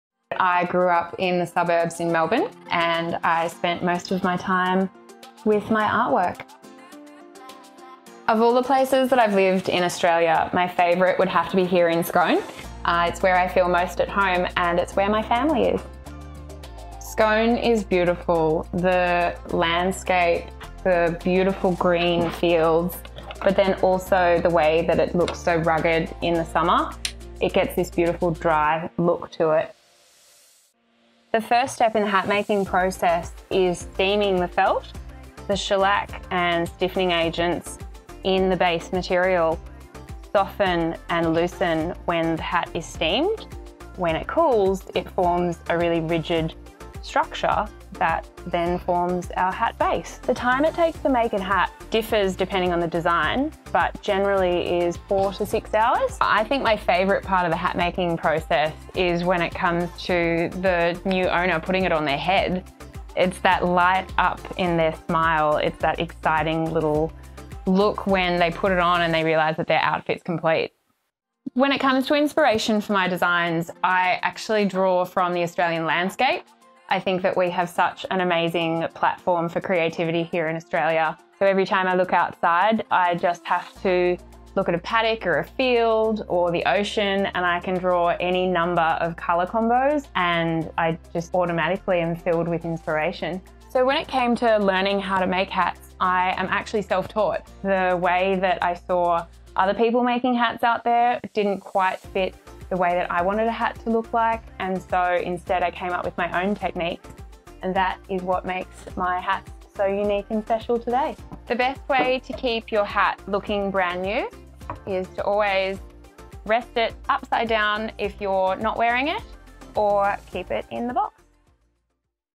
Meet the Maker: Sportscraft x Phylli Hats
Meet milliner and founder of Phylli Designs, Laura Hall.

We've partnered with Laura to create a limited edition range of premium felt hats that will be available at selected stores across Australia.